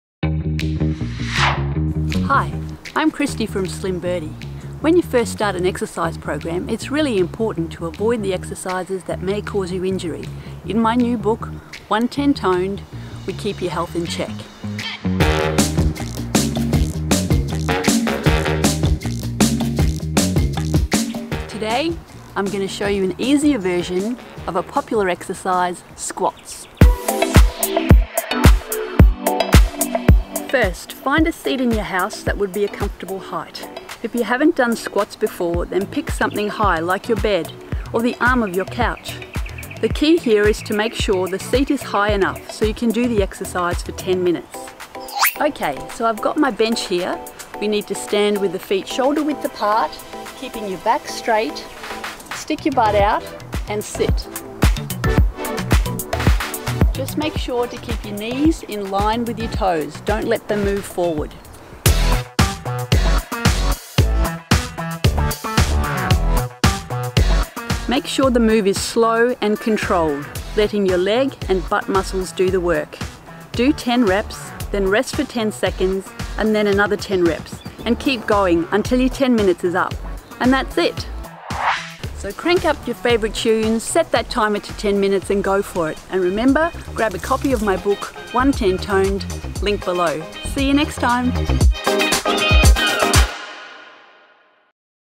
Super star beginners squat exercise
This home workout video demonstrates an easier version of squats in the One Ten Toned home workout program. Great for beginners, low impact and easy on the knees and back. Build muscle tone in only 10 minutes a day, whenever it suits you.

Filmed in Brisbane Australia.

If you need any equipment, here are some options:

Ankle Weights
Yoga Mat
Dumb Bells